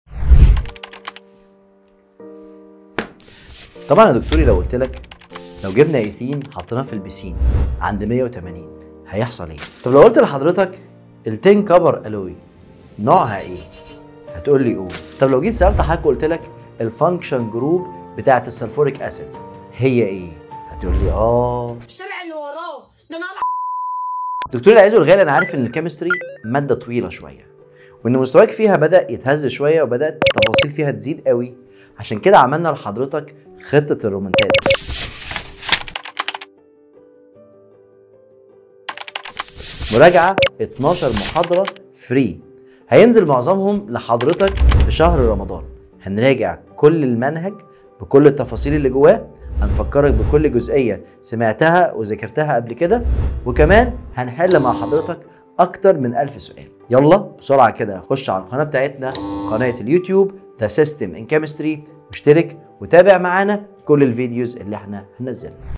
⚠ مراجعة ال Chemistry مجانااا علي قتاة اليوتيوب 🧪🥰
⚠🚨 مراجعة ال Chemistry مجانااا علي قناة اليوتيوب طول شهر رمضان🧪🥰
الصف_الثالث_الثانوي Freeeeeeee
🔵 علشان رمضان شهر الكرم باذن الله قرارنا كل اسبوع
هينزل ليكم مراجعة Chapter كامل 🧪✅
🚨هتحتوي علي الأتي🚨⬇
1️⃣ شرح ال Chapter بالتفاصيل الملل اكنك اول مرة تذكروا🥰
2️⃣ هينزلكم فيديو حل اسئلة علي كل جزئية في Chapter 📝
3️⃣ هينزل فيديو حل اسئلة امتحانات الأعوام السابقة وده مهم جدا جدا ⚠⚠
4️⃣ هيكون في جروب للمتابعة علي الواتساب هينزل في كل ال Pdfs والأسئلة 📙
5️⃣ طبعا هيكون في زوووم مباشر كل فترة علشان نحل الي هيقف معاكم ✅

⚠ كل الي عليك يا دكتوري عشان تتابع كل شرح و الحل مجاني تشترك علي قناة اليوتيوب ⬇

🚨 ومستنيكم في أول محاضرة لينا أول خميس في رمضان بامرالله 🤍
هنجيب الكيمستري في شوال ✅🥰